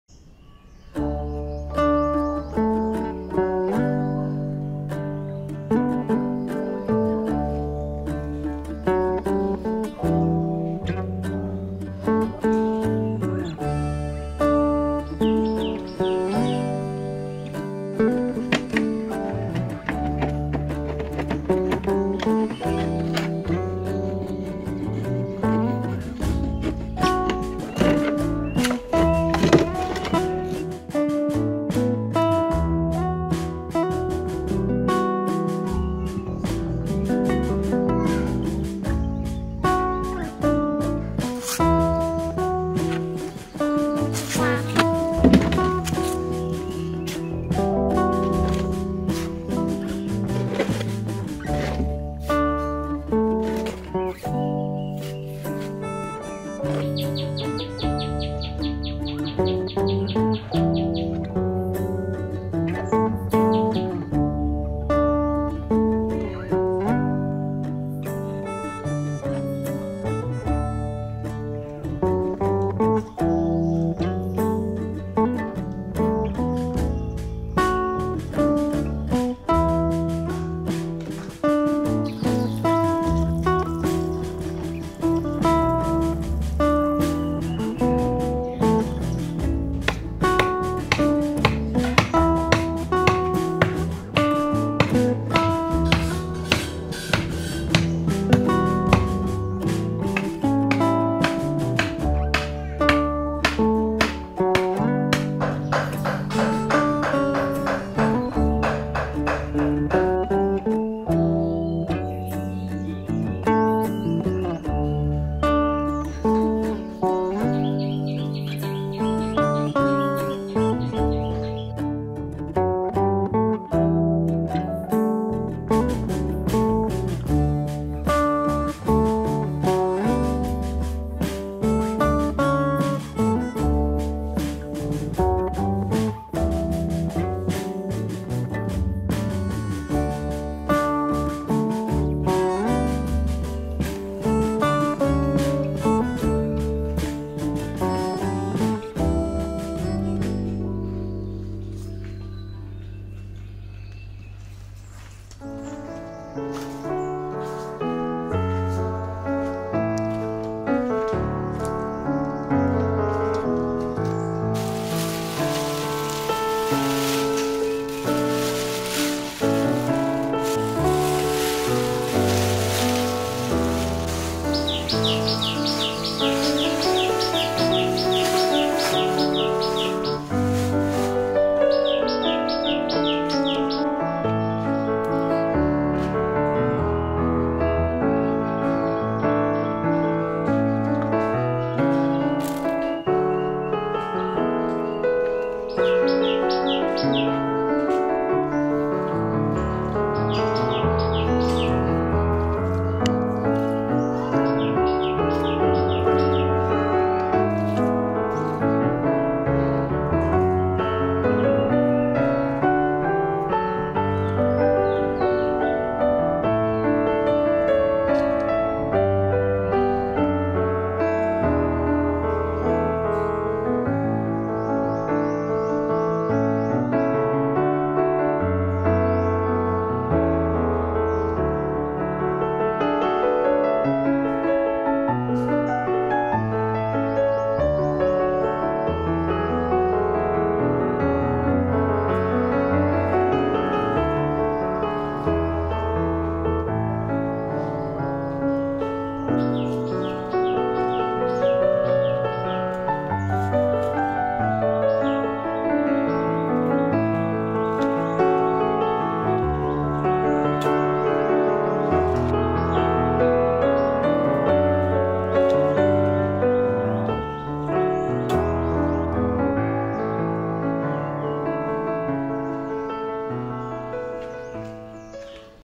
No need for a garden, Growing Clean Vegetables at home is very easy
Chibi teddy bear's daily life || No need for a garden, Growing Clean Vegetables at home is very easy , in this video i grow salad from seedling